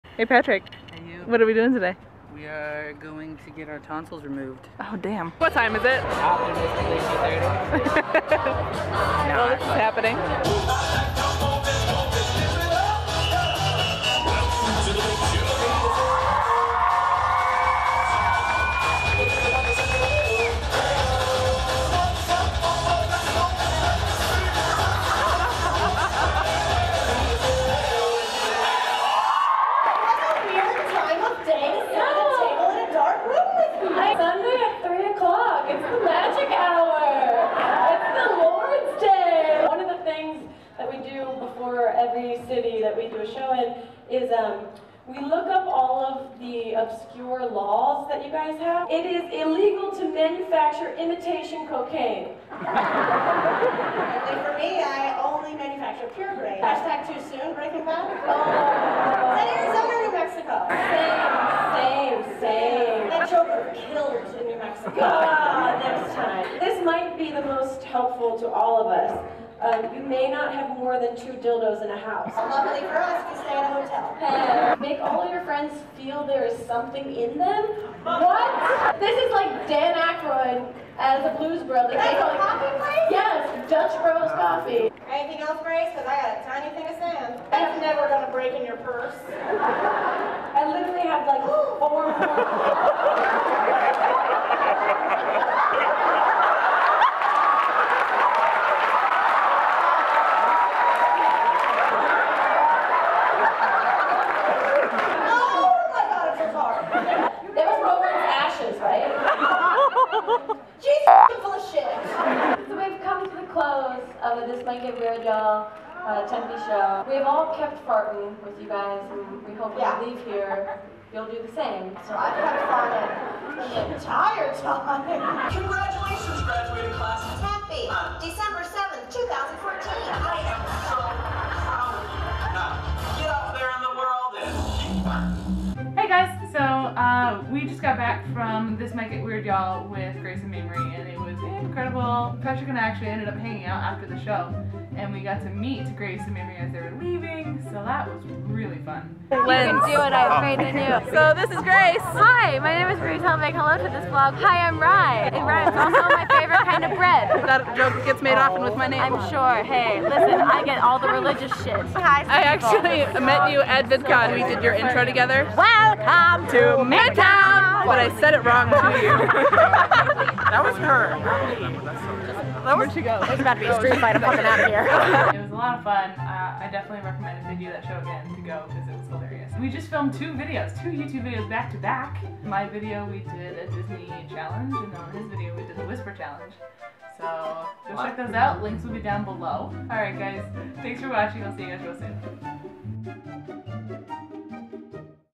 It Got Weird, Ya'll
Saw Grace Helbig and Mamrie Hart's improv comedy show, This Might Get Weird, Ya'll, in my home town this week and it was HILARIOUS!